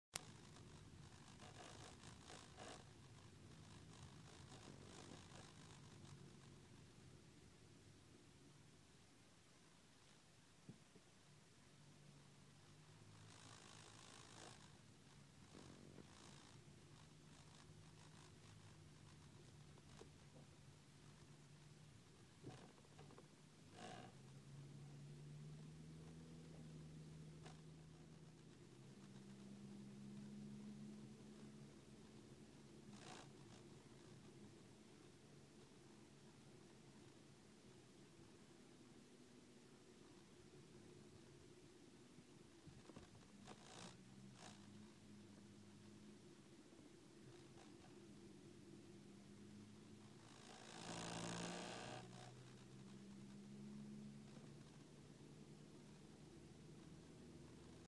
test iPhone 4 onderweg naar Gouda
Test van iPhone en telefoonhouder (laatste is bij deze afgekeurd).